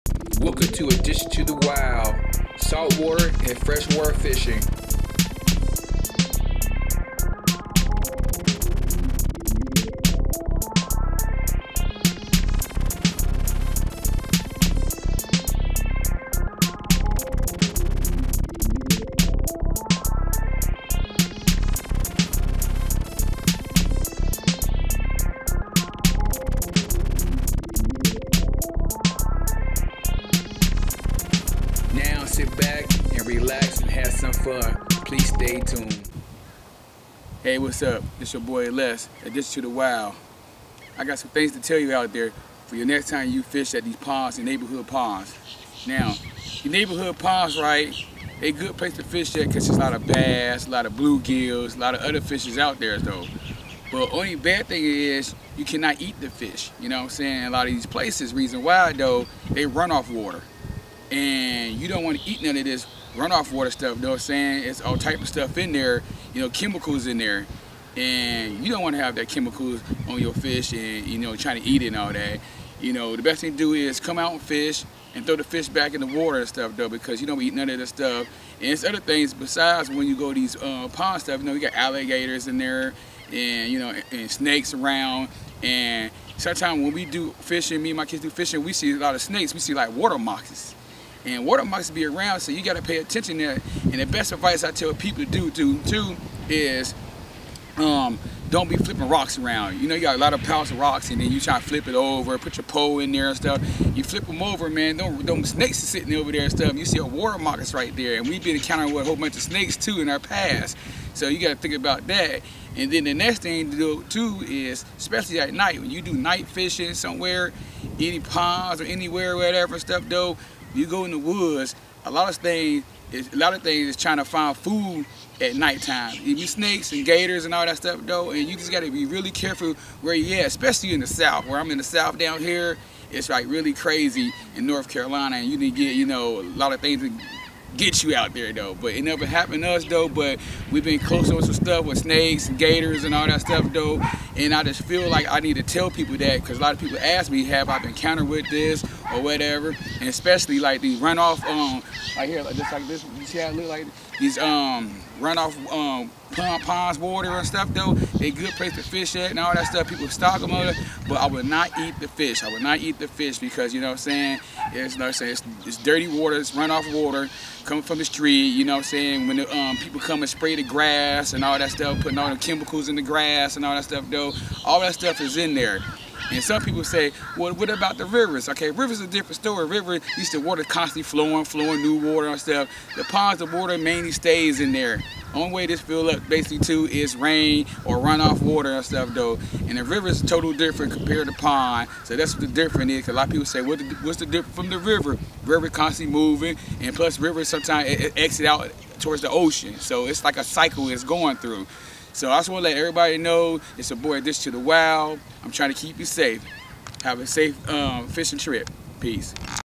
Why you should not eat fish from community ponds
When you take your next trip to the community ponds you might want to see this. I see folks take fish home to eat them. Before you do that you might want to watch this video.